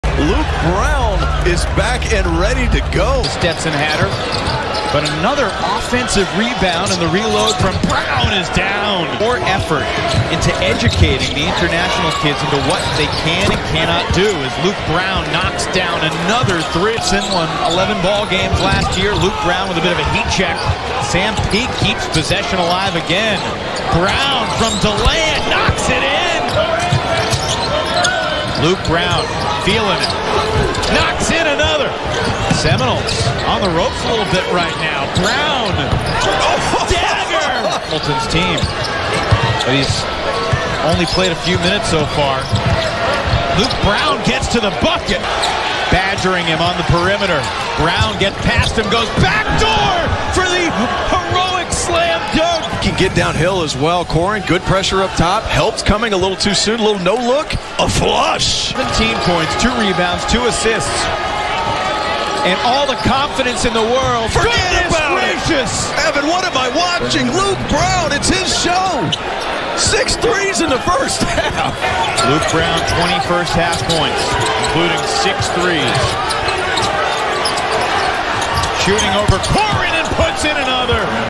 Luke Brown of Stetson hits SIX THREES in the first half vs. Florida State!
Luke Brown of Stetson was frying Florida State last night with 27 points off the bench.